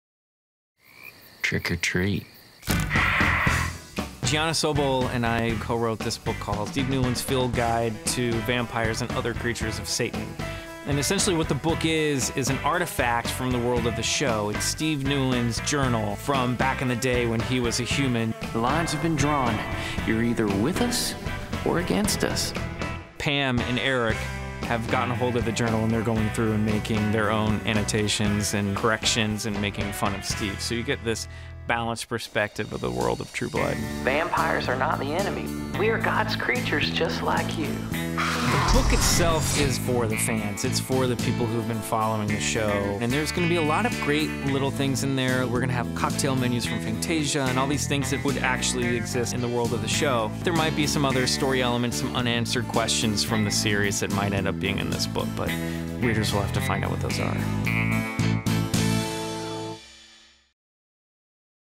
Steve Newlin's Field Guide To Vampires -- Book Trailer
The vampires, werewolves, faeries, and other supernatural creatures that call Bon Temps, Louisiana home aren't universally accepted around town. Sweet-talking, bible-thumping Steve Newlin is their self-appointed nemesis. In his demented field guide, Steve has recorded all he knows about these Creatures of Satan, and that's a lot: their strengths, their weaknesses, their bylaws, and, of course, how to kill them. Sadly for Steve—but luckily for True Blood fans everywhere—his journal was stolen by Fangtasia vampires Pam and Eric, who have defaced it in a most disrespectful and snarky manner. A cunningly produced facsimile, and a hilarious read, The Field Guide to Vampires is packed with untold insight into our favorite TV characters, along with photos, letters, postcards, handwritten notes, and Steve's own idle doodles in a scrapbook that perfectly captures the tongue-in-cheek camp that makes True Blood the most widely watched show on HBO.

 Gianna Sobol is an associate producer for True Blood. She lives in Los Angeles.

Michael McMillian is an actor, writer, illustrator who currently plays Steve Newlin in True Blood. He lives in Los Angeles.